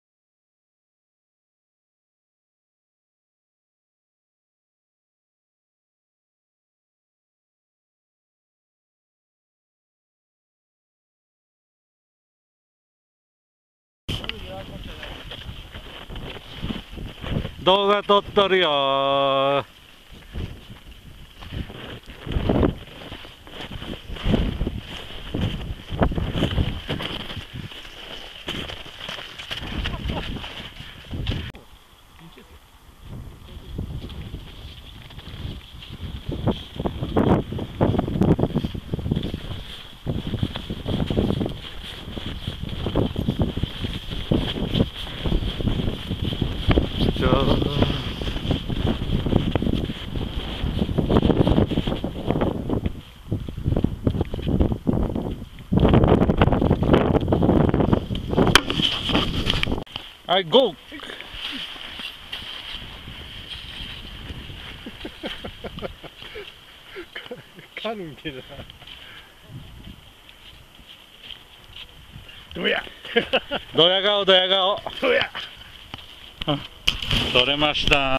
2012.2.12 蓼科山
雪の蓼科山アタックです　下りは尻スキーで下れますよ